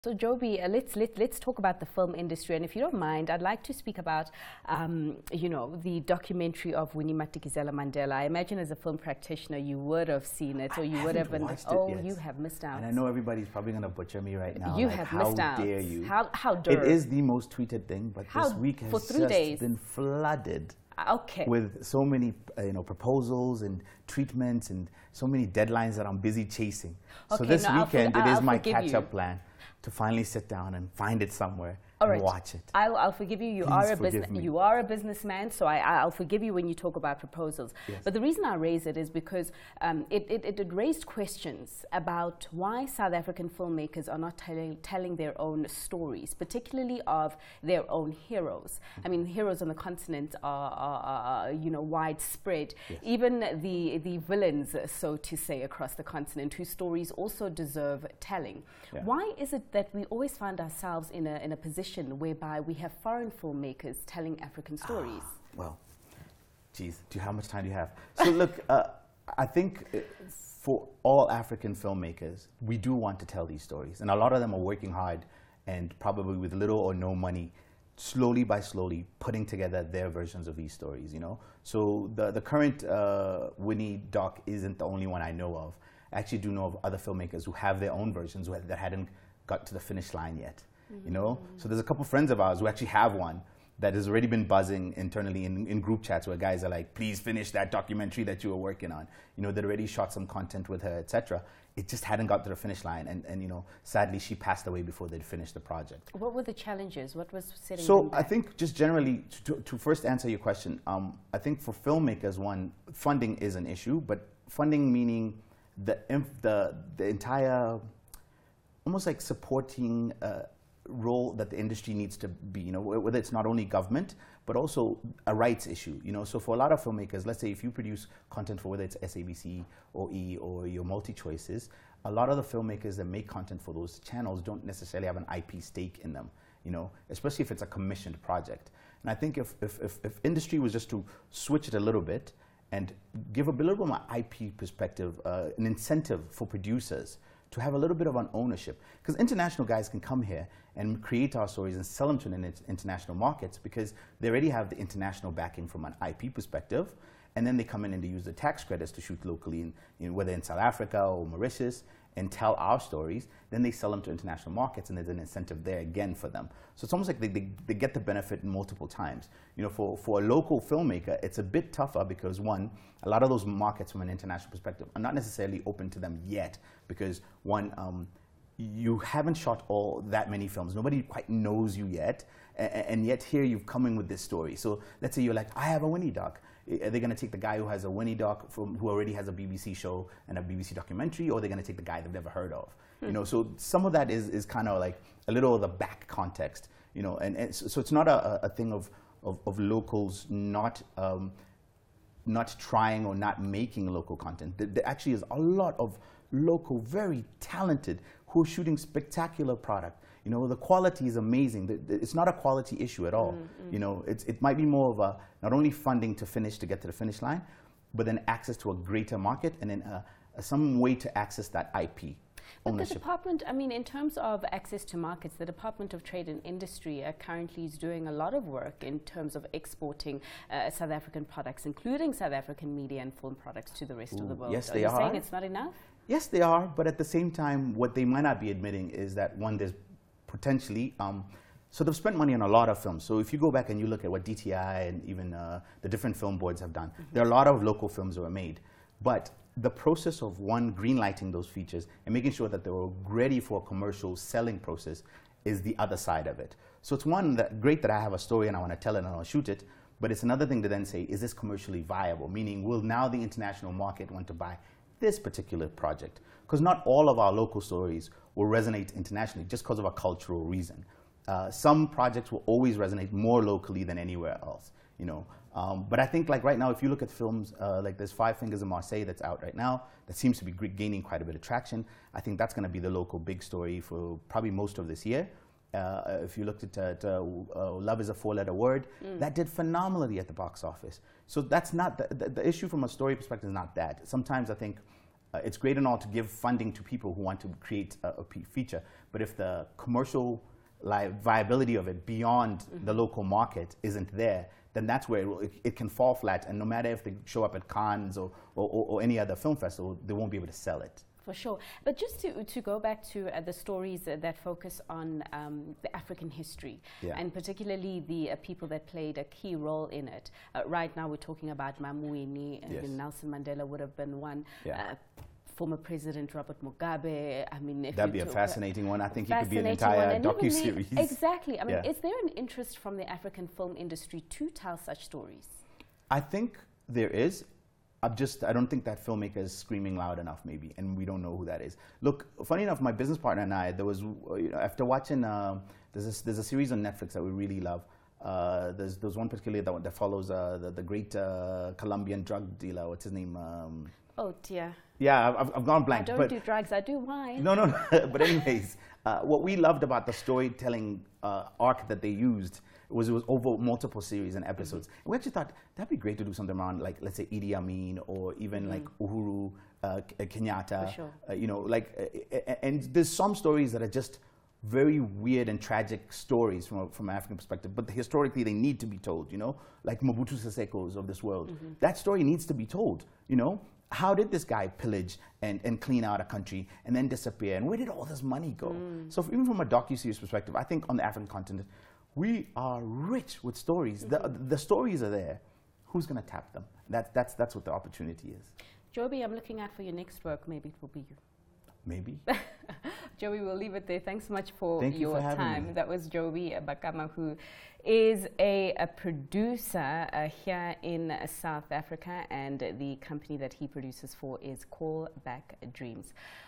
Why foreign film makers not Africans are telling the Africa story
The Gauteng Film Industry is said to contribute R2.5 billion annually towards economic and social growth and is revered worldwide for attracting investment and partnerships.  This has led to ongoing admissions and collaborations with global film festivals including the Canne film festival and the East End film festival. Jobie Bakama, Producer of Callback Dreams, joins CNBC Africa to discuss the film industry as an emerging market in South Africa and his entrepreneurial journey.